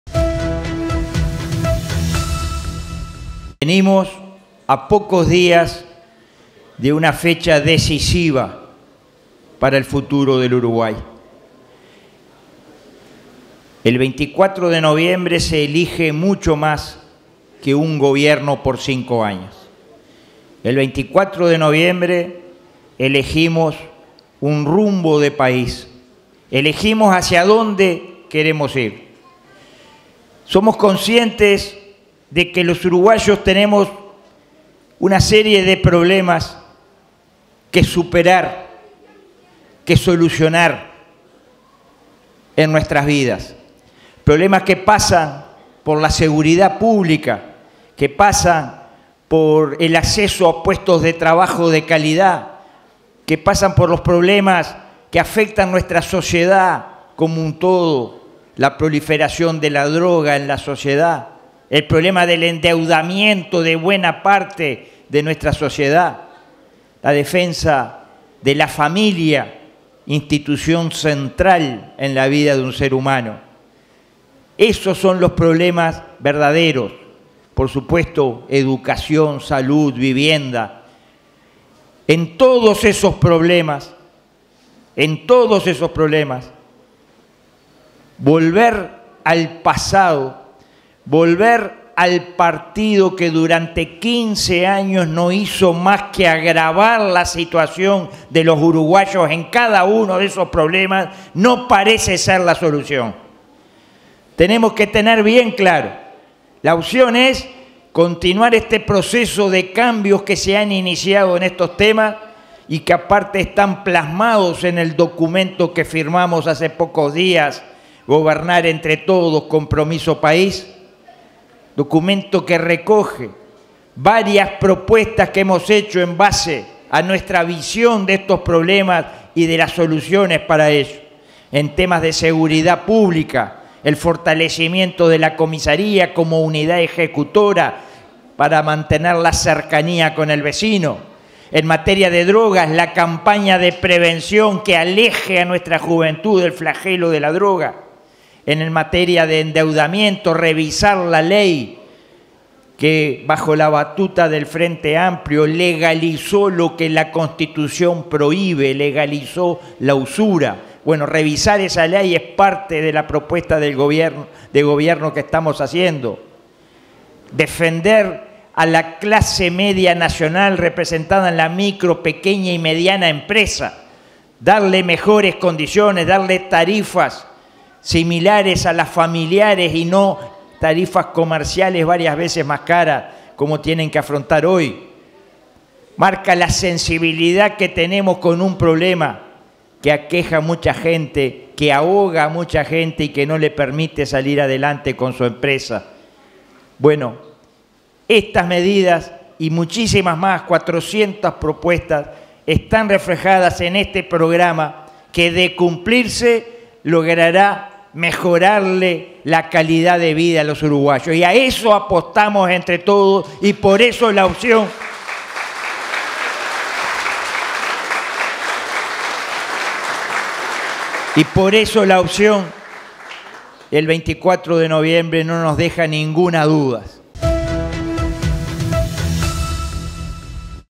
Guido Manini Rios en Tacuarembó
Habló en el acto de la coalición en Tacuarembó